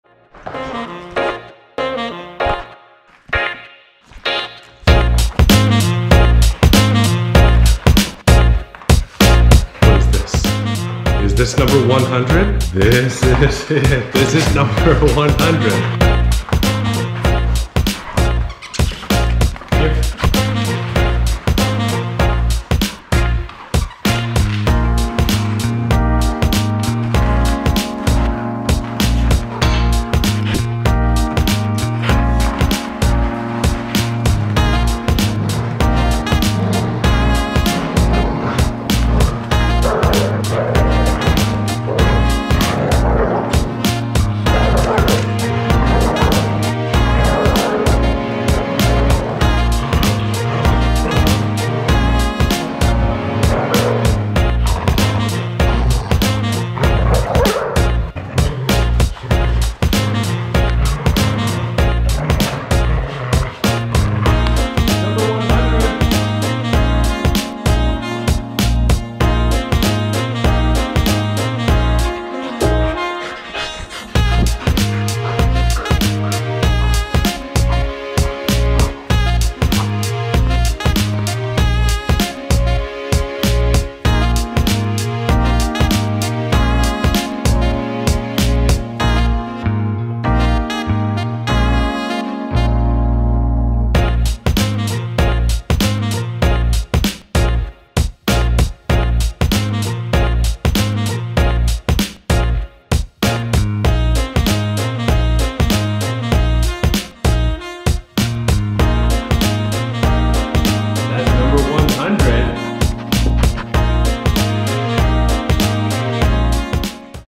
They Love to Destroy Toys
Another one bites the dust. Thor and Storm enjoying destroying yet another toy. This one got them to 100 destroyed toys